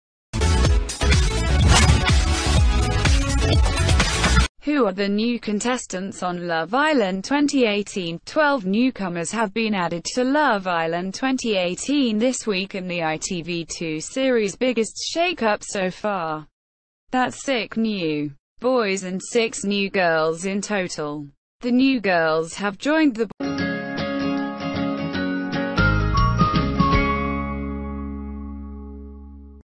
Love Island 2018: Who are the new boys and girls entering Casa Amor?
► Who are the new contestants on Love Island 2018?Twelve newcomers have been added to Love Island 2018...